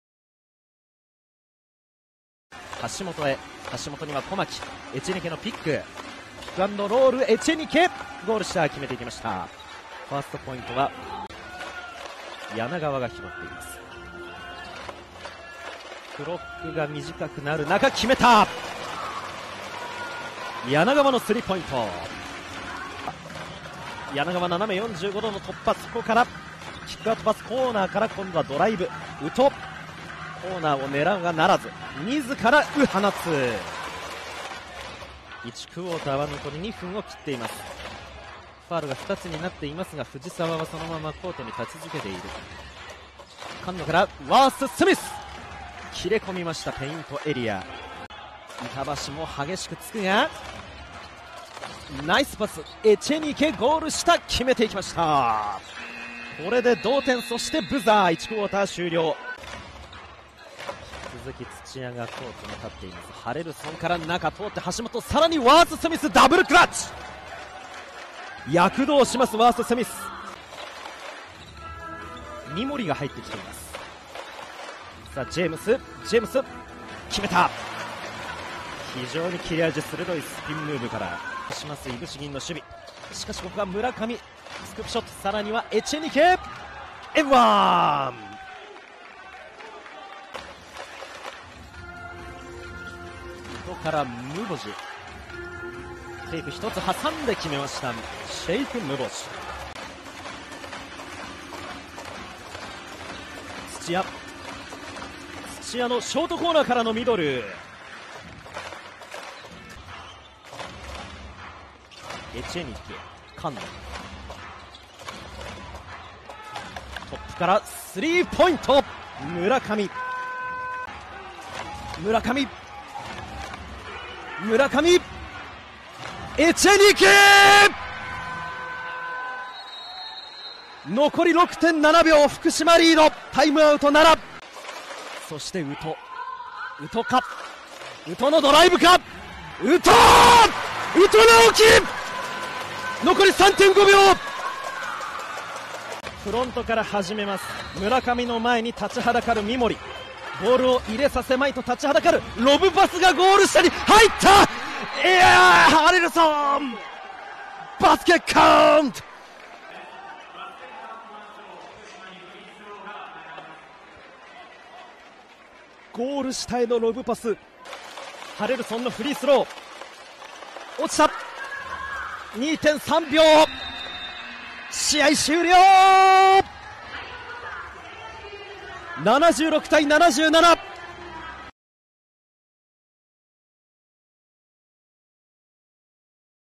Bambitious Nara vs. Fukushima Firebonds - Game Highlights
Watch the Game Highlights from Bambitious Nara vs. Fukushima Firebonds, 02/12/2023

00:00:00 - 1st Quarter
00:00:59 - 2nd Quarter
00:01:21 - 3rd Quarter
00:01:40 - 4th Quarter